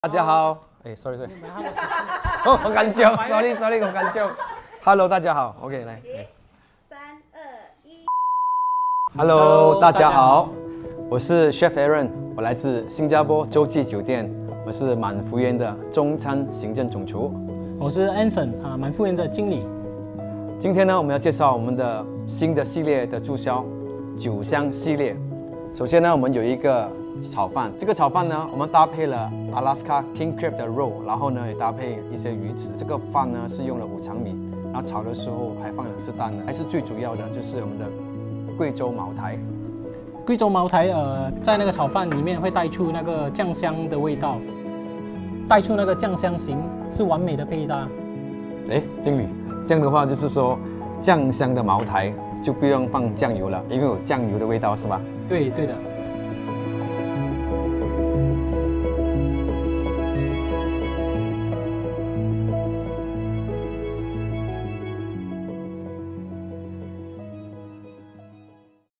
Man Fu Yuan Spirit-Infused Epicurean Delights
Dive into an extraordinary culinary experience with Chef Aaron and Anson, restaurant manager of @manfuyuansg, as they introduce our exquisite Alaskan Crab Fried Rice with Maotai and Tobiko.

Watch as Chef @chefaarontan masterfully combines the rich flavors of Alaskan crab with the complex notes of Maotai, all topped with the delightful crunch of tobiko. The experience is taken to new heights with a shot of Kweichow Moutai, creating a luxurious and unforgettable finish.

Join us for a taste of Spirit-Infused Epicurean Delights from now until 31 July, where tradition meets innovation in every bite.

🌐 Visit manfuyuan.sg for more information.